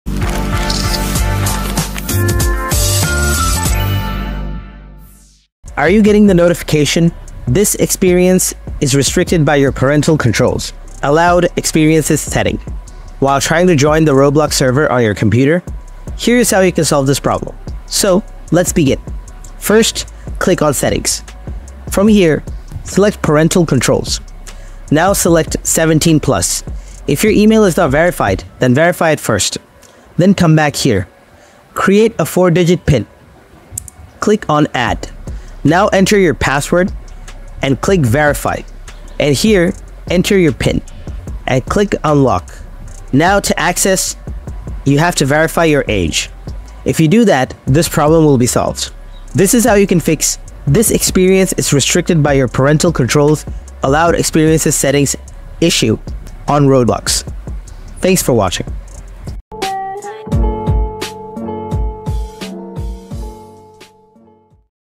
Fix "This experience is restricted by your parental controls" Error in Roblox
Do you keep getting Roblox this experience is restricted by your parental controls error. Watch this video to fix this experience-restricted error in Roblox.
Recently many Roblox users have been saying that they are getting this experience is restricted by your parental controls allowed experiences setting error while trying to join the Roblox server. Now if you also facing that problem his experience is a restricted error in Roblox then don't worry!
After analyzing the issue, I will show you how to Fix Roblox this experience is restricted by your parental controls.

00:01- Video intro
00:19- Main Process
00:55- Process Ending